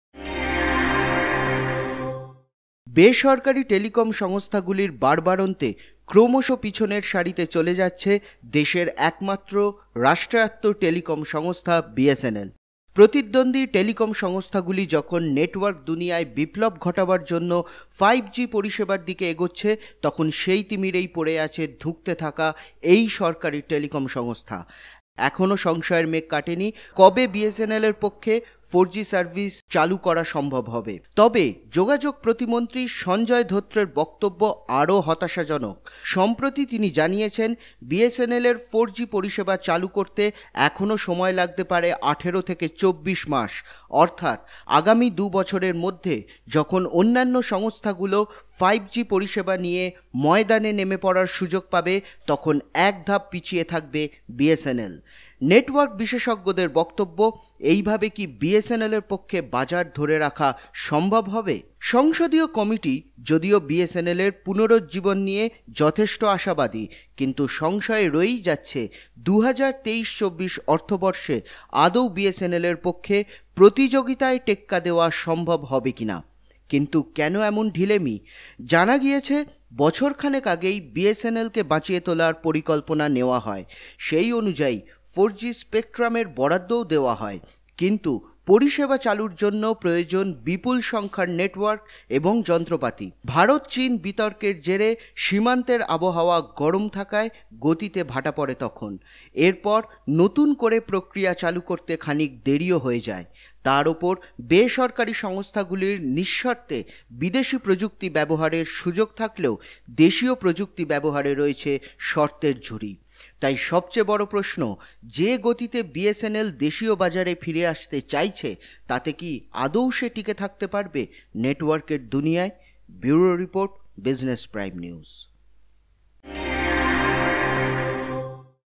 Business Prime News - BSNL still behind 4G Belt - বিএসএনএলের 4G পরিষেবা চালু হতে এখনও দেড় বছর
বি পি এন ডেস্কঃ বেসরকারি টেলিকম সংস্থাগুলির বাড়বাড়ন্তে ক্রমশ পিছনের সারিতে চলে যাচ্ছে দেশের একমাত্র রাষ্ট্রায়ত্ত টেলিকম সংস্থা বিএসএনএল। প্রতিদ্বন্দ্বী টেলিকম সংস্থাগুলি যখন নেটওয়ার্ক দুনিয়ায় বিপ্লব ঘটাবার জন্য ৫জি পরিষেবার দিকে এগোচ্ছে তখন সেই তিমিরেই পড়ে আছে ধুঁকতে থাকা এই সরকারি টেলিকম সংস্থা। এখনও সংশয়ের মেঘ কাটে নি, কবে বিএসএনএলের পক্ষে ৪জি সার্ভিস চালু করা সম্ভব হবে। তবে যোগাযোগ প্রতিমন্ত্রী সঞ্জয় ধোত্রের বক্তব্য আরও হতাশাজনক। সম্প্রতি তিনি জানিয়েছেন, বিএসএনএলের ৪জি পরিষেবা চালু করতে এখনও সময় লাগতে পারে ১৮ থেকে ২৪ মাস! অর্থাৎ আগামী দু’বছরের মধ্যে যখন অন্যান্য সংস্থাগুলো ৫জি পরিষেবা নিয়ে হয়ত ময়দানে নেমে পড়ার সুযোগ পাবে তখন একধাপ পিছিয়ে থাকবে বিএসএনএল। নেটওয়ার্ক বিশেষজ্ঞদের বক্তব্য, এইভাবে কি বিএসএনএলের পক্ষে বাজার ধরে রাখা সম্ভব হবে?

সংসদীয় কমিটি যদিও বিএসএনএলের পুনরুজ্জীবন নিয়ে যথেষ্ট আশাবাদী, কিন্তু সংশয় রয়েই যাচ্ছে যে ২০২৩-২৪ অর্থবর্ষে আদৌ বিএসএনএলের পক্ষে প্রতিযোগিতায় টেক্কা দেওয়া সম্ভব হবে কিনা। কিন্তু কেন এমন ঢিলেমি?

জানা গিয়েছে, বছরখানেক আগেই বিএসএনএলকে বাঁচিয়ে তোলার পরিকল্পনা নেওয়া হয়। সেই অনুযায়ী ৪জি স্পেকট্রামের বরাদ্দও দেওয়া হয়। কিন্তু পরিষেবা চালুর জন্য প্রয়োজন বিপুল সংখ্যার নেটওয়ার্ক এবং যন্ত্রপাতি। ভারত-চিন বিতর্কের জেরে সীমান্তের আবহাওয়া গরম থাকায় গতিতে ভাটা পড়ে তখন। এরপর নতুন করে প্রক্রিয়া চালু করতে খানিক দেরিও হয়ে যায়। তার ওপর বেসরকারি সংস্থাগুলির নিঃশর্তে বিদেশি প্রযুক্তি ব্যবহারের সুযোগ থাকলেও দেশীয় প্রযুক্তি ব্যবহারে রয়েছে শর্তের ঝুড়ি।

তাই সবচেয়ে বড় প্রশ্ন, যে গতিতে বিএসএনএল দেশীয় বাজারে ফিরে আসতে চাইছে, তাতে কি আদৌ সে টিকে থাকতে পারবে নেটওয়ার্কের দুনিয়ায়?

ব্যুরো রিপোর্ট, বিজনেস প্রাইম নিউজ